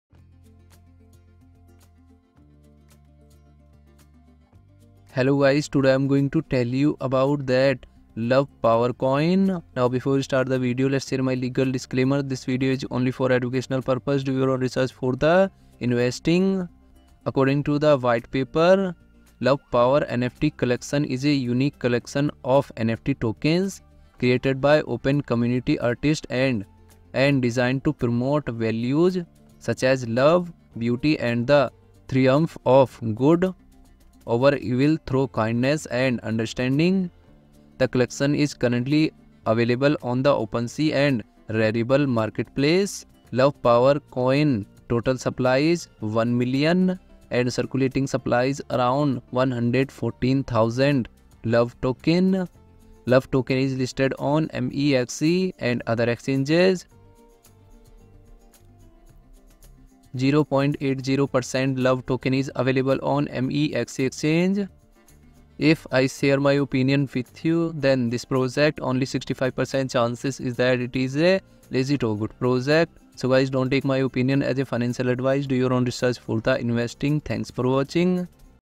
What is Love Power Coin (LOVE) Coin | Review About LOVE Token
In this video, I will share a Review of the Love Power Coin (LOVE) token Crypto Project. I have collected all the data from online sources please don't take this video as financial advice. I am not a financial advisor.